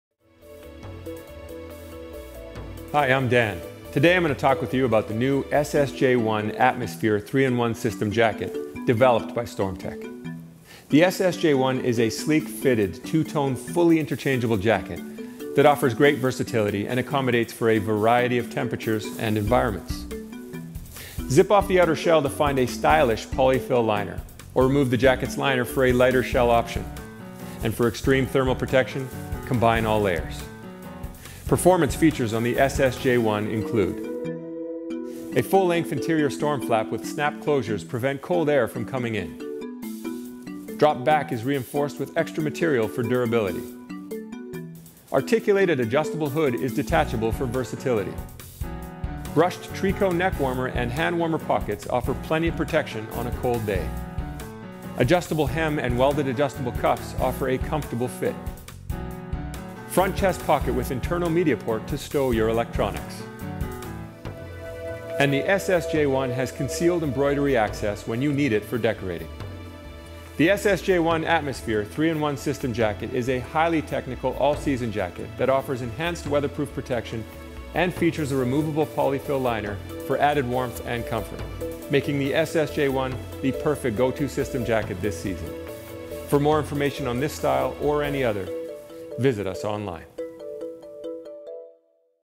Atmosphere 3 in 1 Jacket
The Atmosphere Jacket from Stormtech packs a punch when it comes to knocking out the cold. Designed for optimal versatility & function, the Atmosphere 3-in-1 system jacket, with zip-out poly fill liner, provides superior breathable waterproof protection & warmth. Available from House of Uniforms Melbourne.